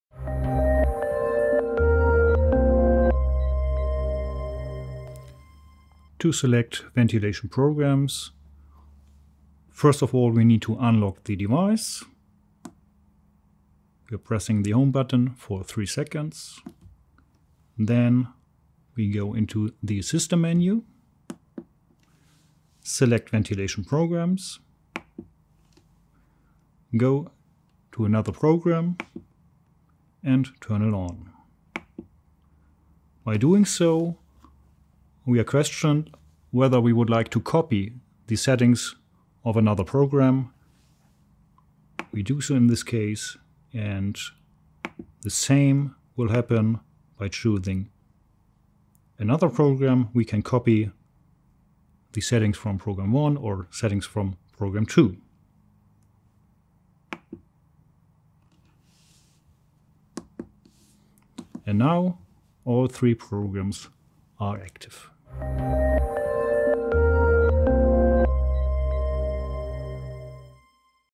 04 prisma VENT50-C Unlocking Programs
In this video we show you the different programs of the prisma VENT50-C ventilator and how to activate them.